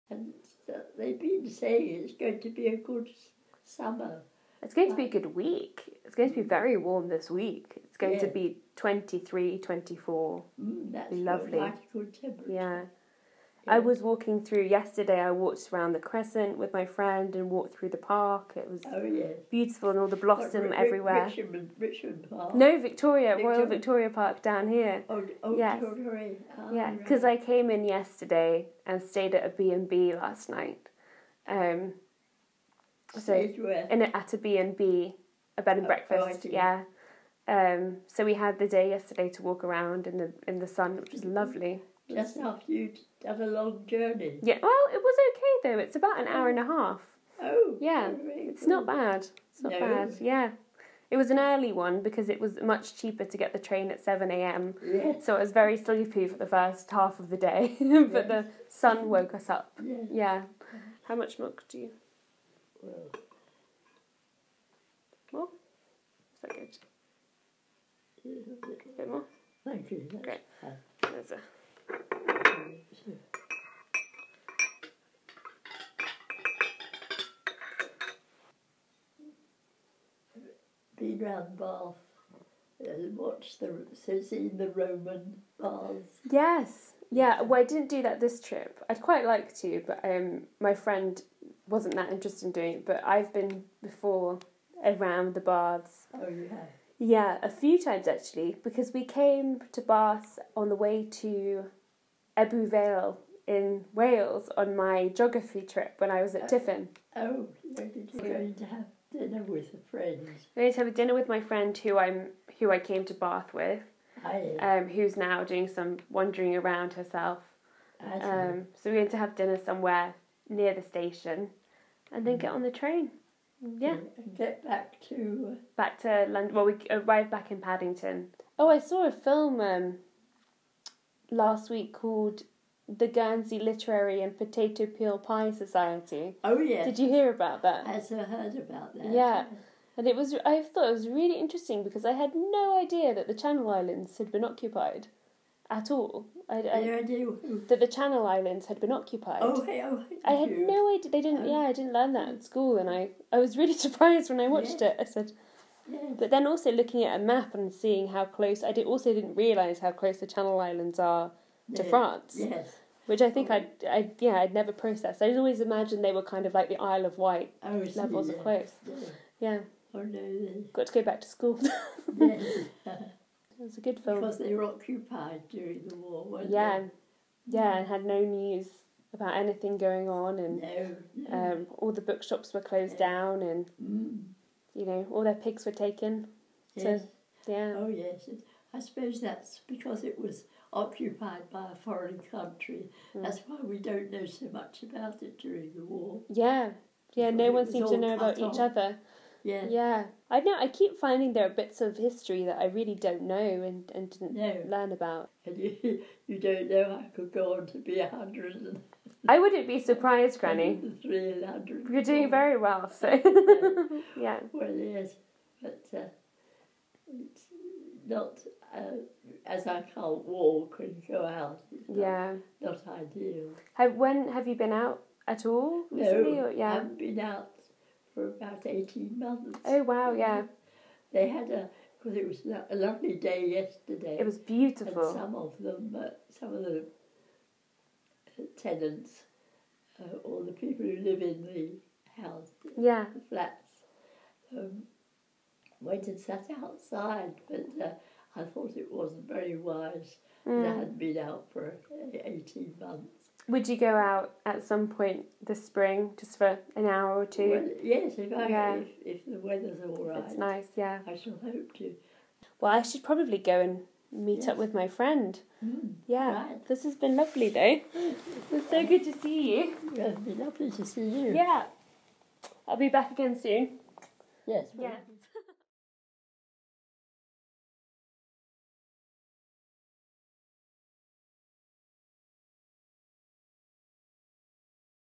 they've been saying it's going to be a good summer (bath 2018)
In spring 2018, Sanne and I went to Bath for the weekend to walk around and also so that I could visit my granny, who at the time was living in a care home there. I had just moved back from living in the USA, so I hadn't been able to see her as much as I'd have liked to, but I've always loved our conversations and had started to record them. Here's that trip, which was the last time I saw her.

Places we visit in this video:
Royal Victoria Park
The Royal Crescent
Sally Lunn's Buns
I *think* The Bath Bun but might be another tea shop
Roman Baths
Mr B's Emporium
Topping & Company books

I also mention the film The Guernsey Literary and Potato Peel Pie Society, which I made a sponsored video about in 2018 You can tell from this video that it was fresh on my mind.